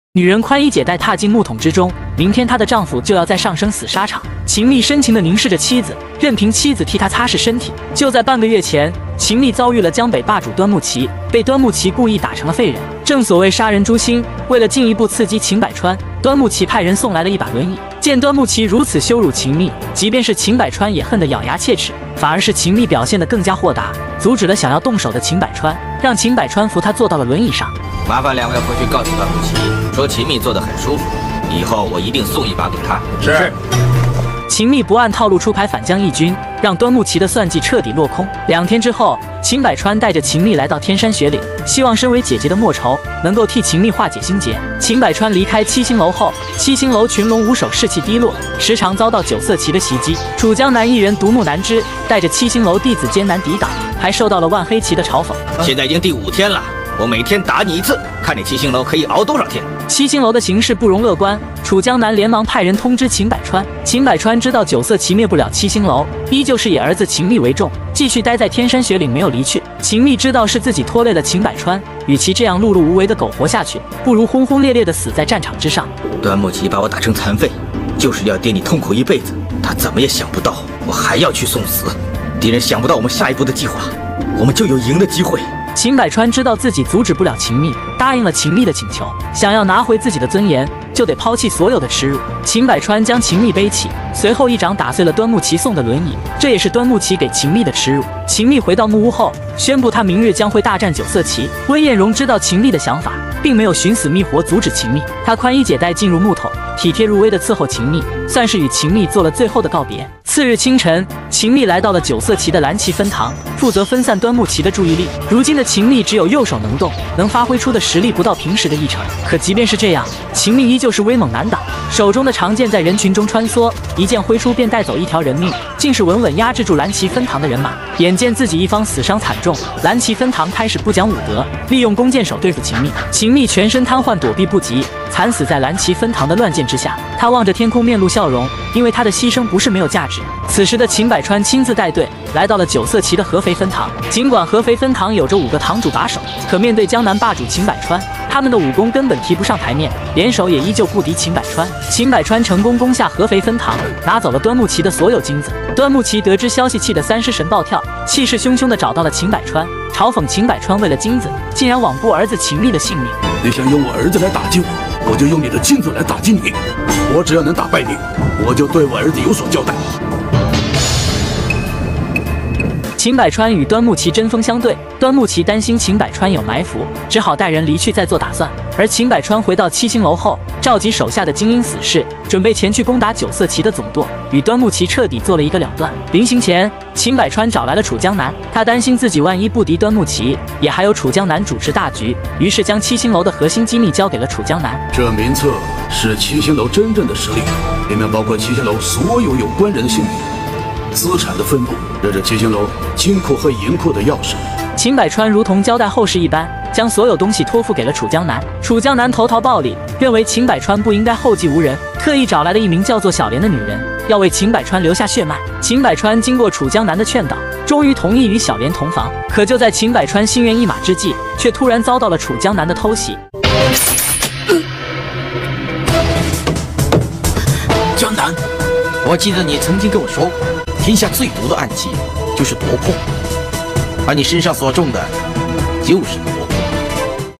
秦汨不愿苟活前去攻打九色旗分堂，秦百川端掉了端木旗的小金库！
秦汨不愿苟活前去攻打九色旗分堂，秦百川端掉了端木旗的金库！